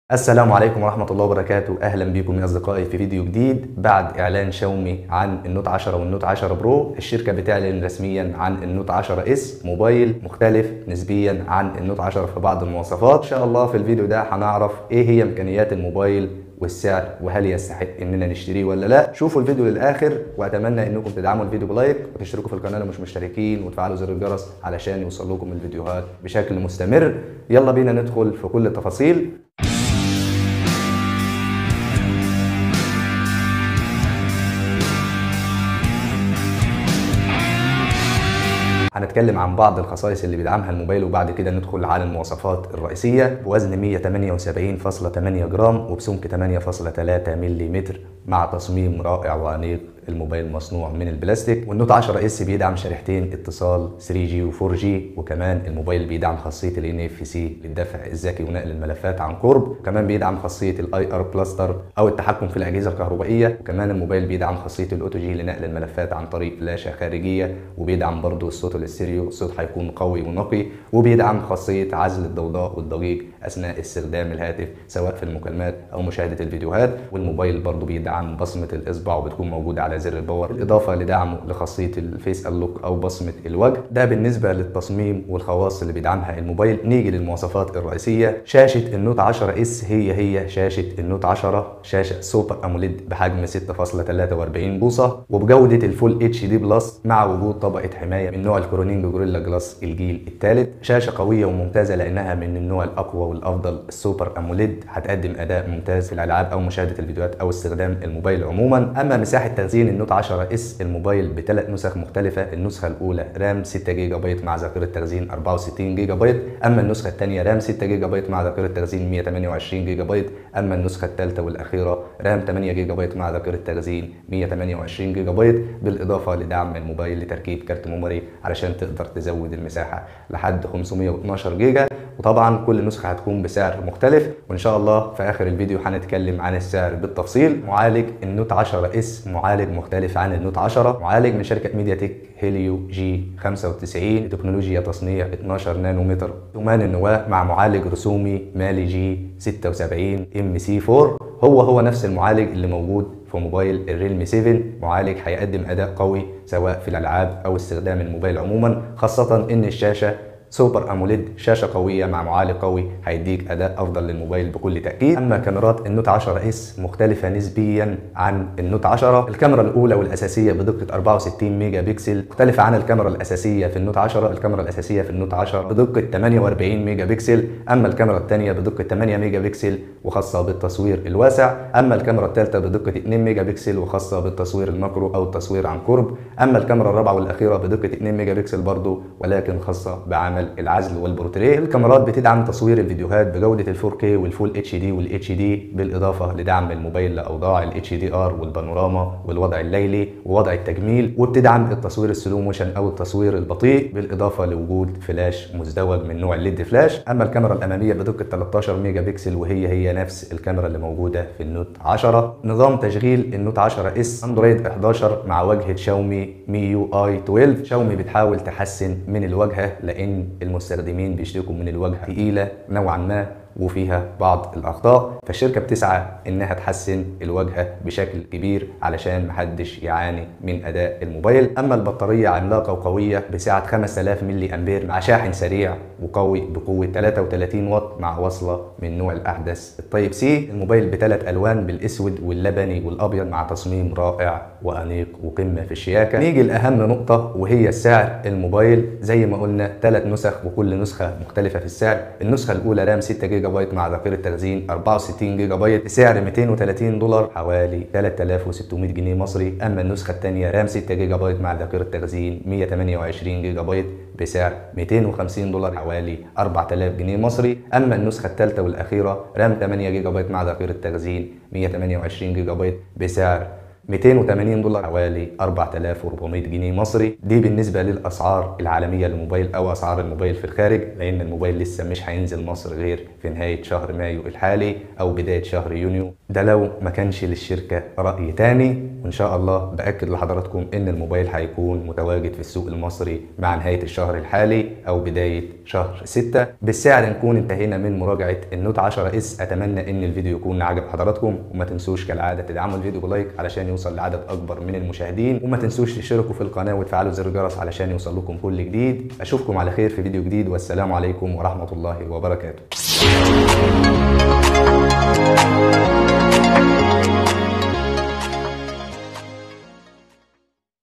شاومي لعبتها صح! | Xiaomi Redmi Note 10s | سعر ومواصفات شاومي نوت 10 اس
أعلنت شركة شاومي عن هاتفها الجديد الـ Xiaomi Redmi Note 10s فهو نسخة محسنة قليلاً من الـ Xiaomi Note 10 حيث تم زيادة دقة الكاميرات بشكل مميز مع تغيير المعالج ليصبح بمعالج أفضل للألعاب وهو ميدياتيك Helio G95 مع الحفاظ على نفس مميزات الـ Redmi Note 10 الأخرى مثل سعة البطارية الكبيرة ونوع الشاشة فالشركة تطمح إلى جذب أكبر عدد ممكن من المستخدمين في الوقت الحالي فسلسلة الـ Redmi من شاومي أصبحت الأكثر إنتشاراً في السوق المصري بفضل المواصفات القوية جداً مع الحفاظ على السعر الرخيص فالشركة تسعى إلى حصول مستخدميها على هواتف تليق بعام 2021 وفي نفس الوقت بسعر رخيص

مراجعة هاتف
⬅️ لينك الفيديو 👇

وسائل التواصل:
Facebook 👇 فيسبوك